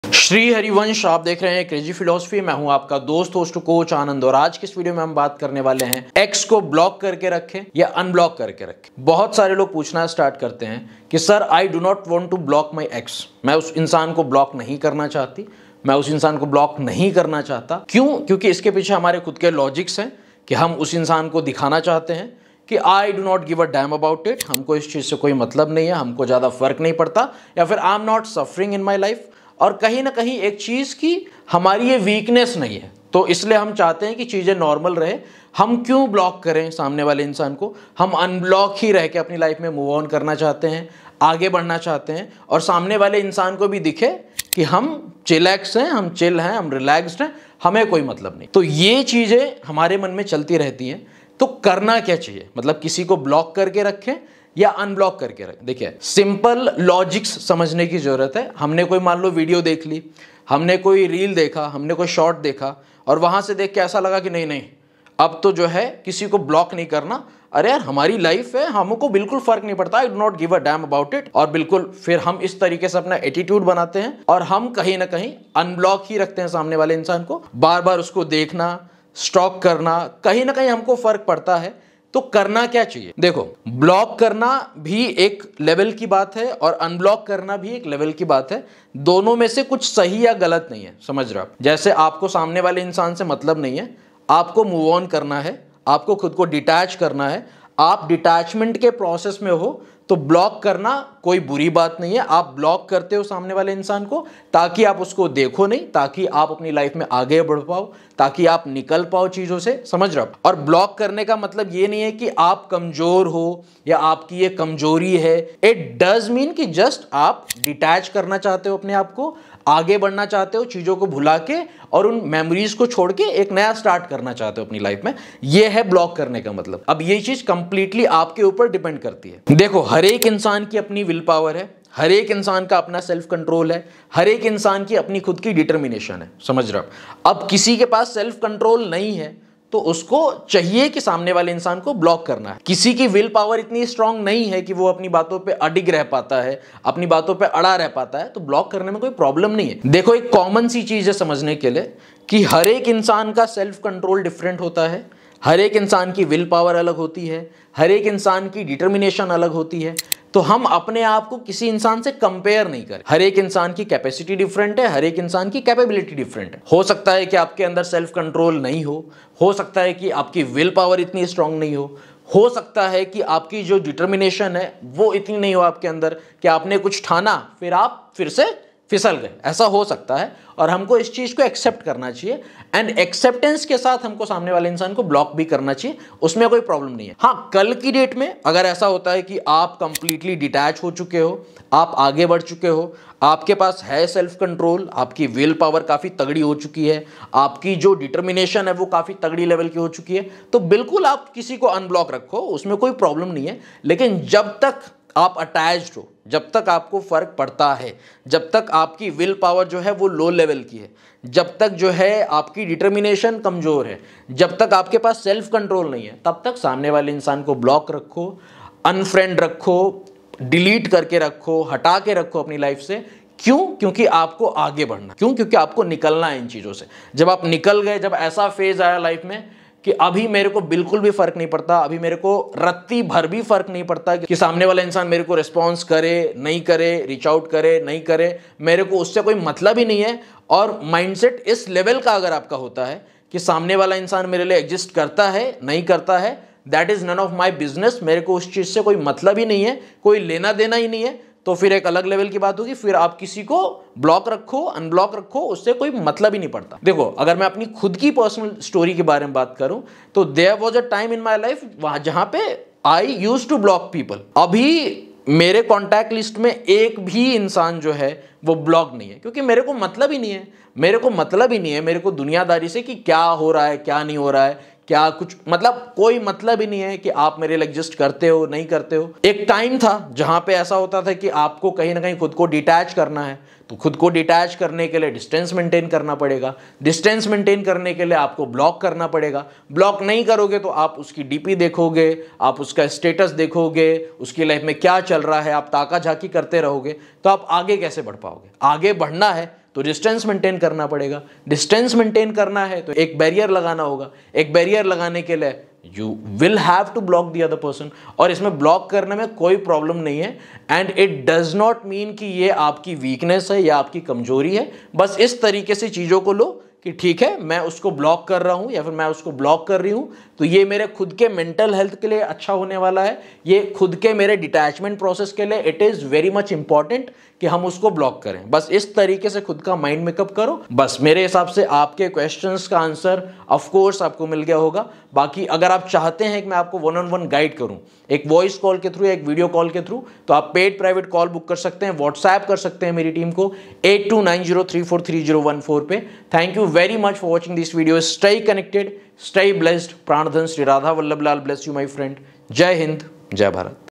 I Do Not Want To Block My Ex? (HERE IS WHAT YOU CAN DO)
In this video Crazy Philosopher has talked about blocking your Ex vs. unblocking him/her. What to do about it and what would be the best decision to make.